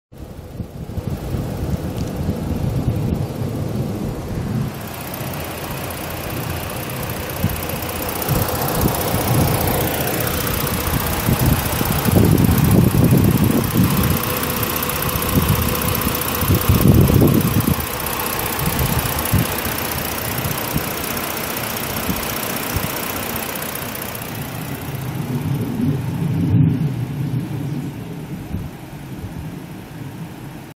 2013 Ford Explorer Police Interceptor SUV for sale at auction | bidding closes August 21, 2018
2013 Ford Explorer Police Interceptor SUV
120,067 miles on odometer
3.7L V6 DOHC 24V gas engine
Automatic transmission
Tilt
Cruise
AM/FM
Power windows, locks, mirrors, and seat
Cloth interior
245/55R18 tires
Missouri title
1FM5K8AR6DGA68551
It's located in Union, MO, and we're selling it to the highest bidder regardless of price.